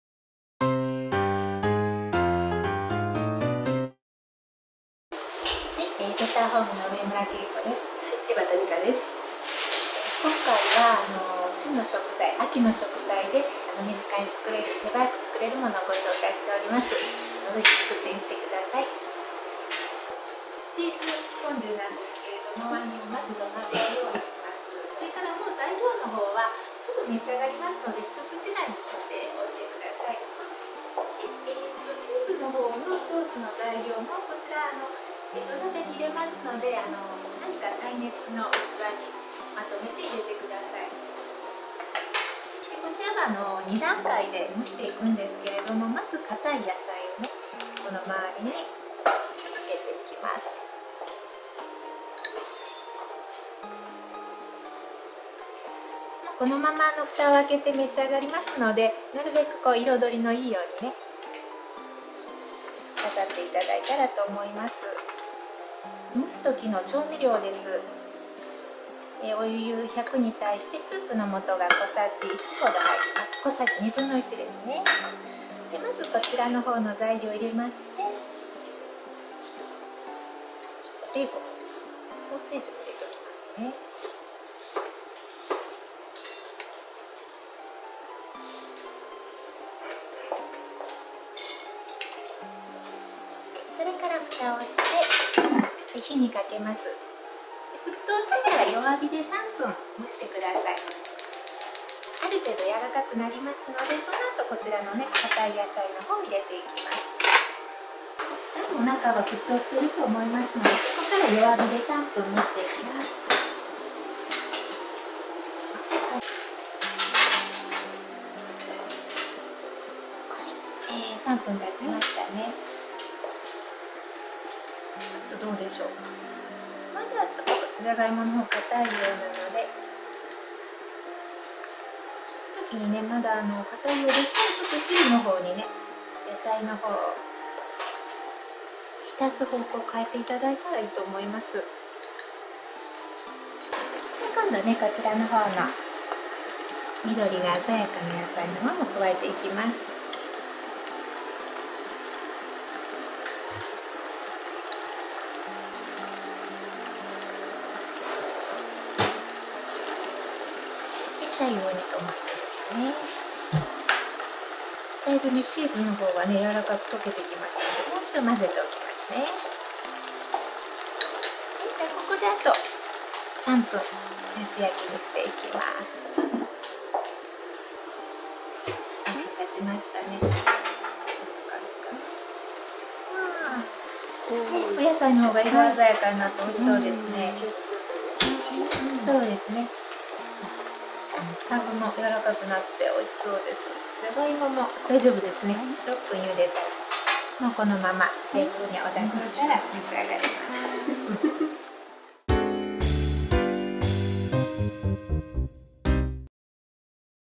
【料理動画】プロの簡単夕食レシピ『土鍋蒸しチーズフォンデュ』
人気のベターホーム協会講師・植村圭子、江端美賀先生が紹介します。